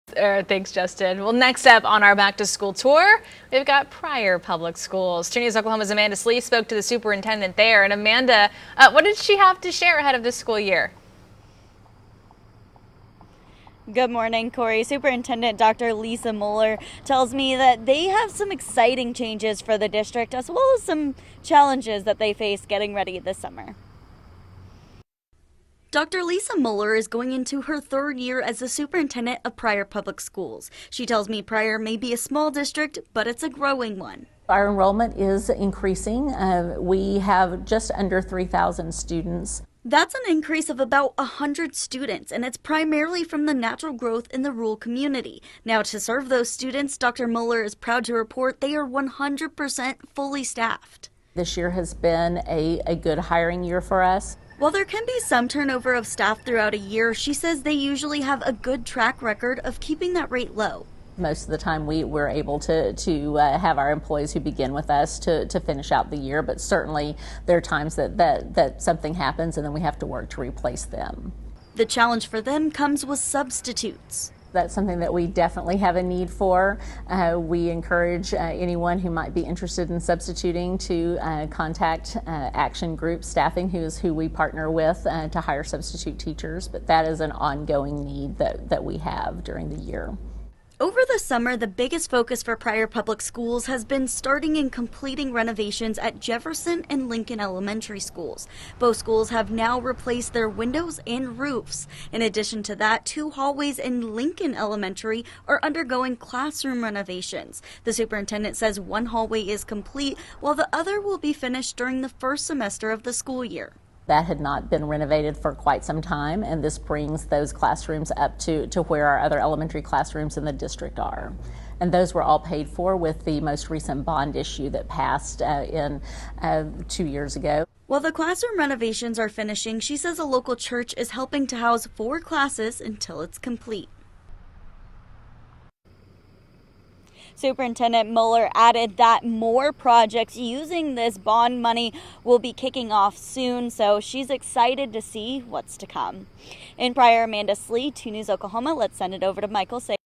Pryor superintendent discusses renovations, enrollment, and staffing ahead of first day
Pryor Public Schools are one of the next districts to go back on Thursday.

Superintendent Dr. Lisa Muller spoke with 2 News ahead of the first day. She shared some exciting changes to the district as well as some challenges they've faced in getting ready for the school year.

Dr. Muller is going into her third year as the superintendent of Pryor Public Schools.

2 News' Amanda Slee reports.